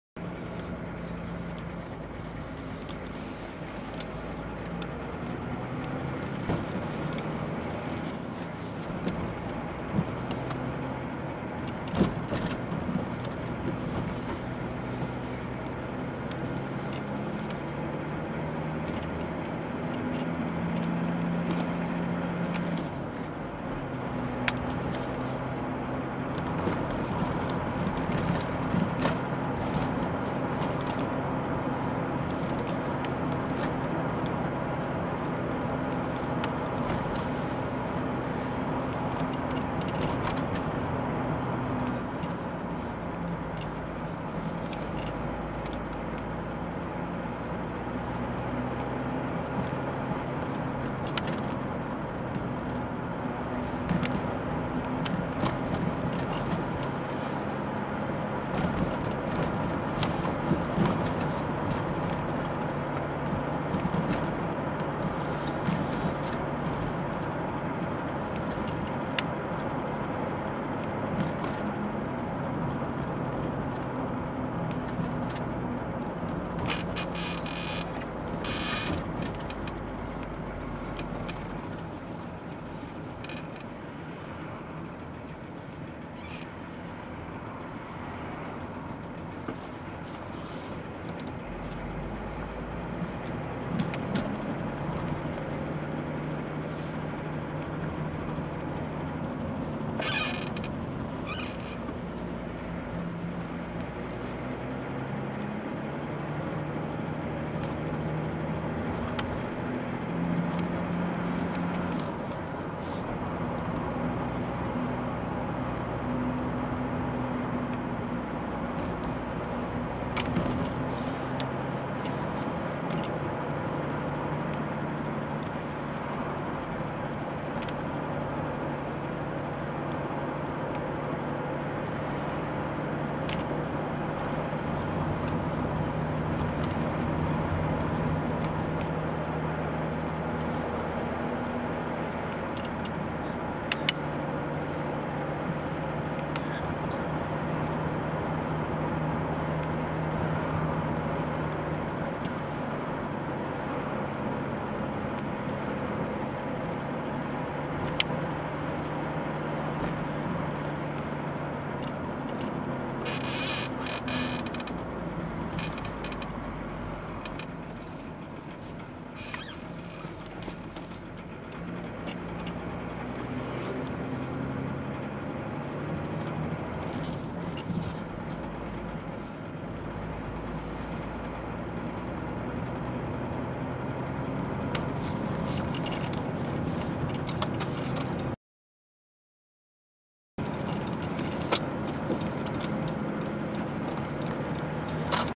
Driving Along Sidbury & London Road A44, Worcester, Worcestershire, England 15th October 2010
Driving along Sidbury & London Road (A44), Worcester, Worcestershire, England
Videoed on Friday, 15th October 2010